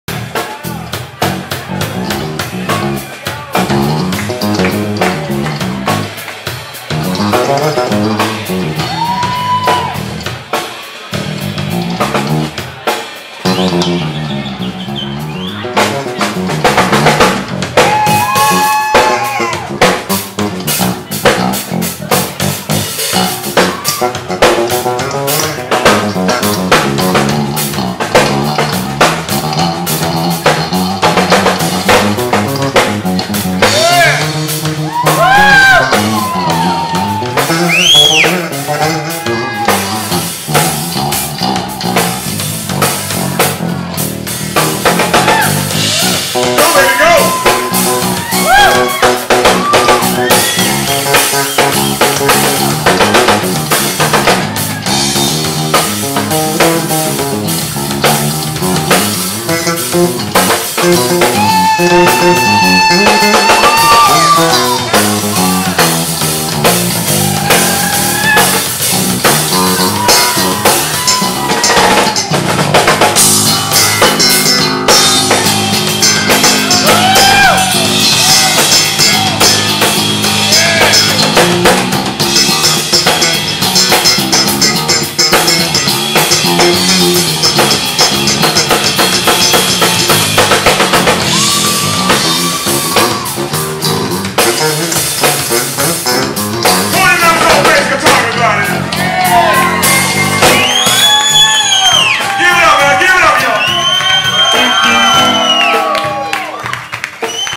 Cornelius Mims 'LIVE' Bass Solo w Dennis Jones Band @Torch Club
3/09/19 Corney Mims bringin the Bass you can Rock to!
Guitar:  Dennis Jones
Bass:  Cornelius Mims
Drums:  Raymond Johnson